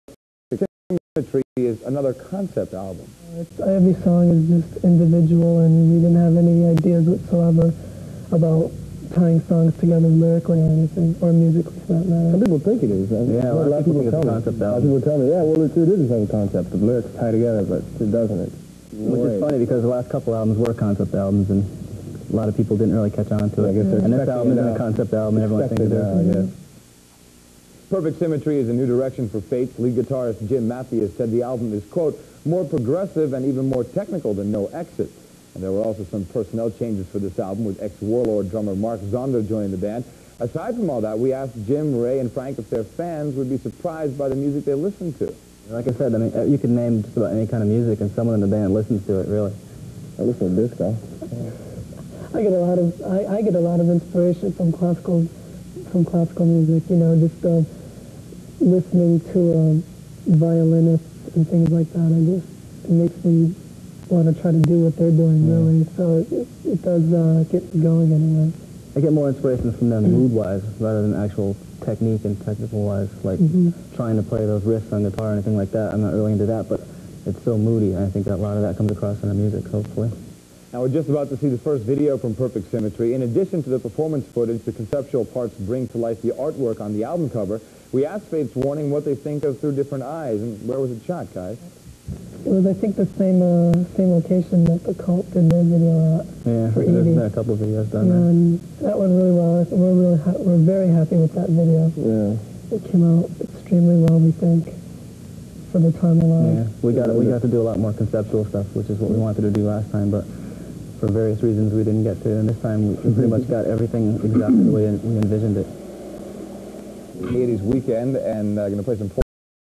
Fates Warning MTV interview 1989
Jim Matheos, Ray Alder & Frank Aresti
MTV studios in NYC 1990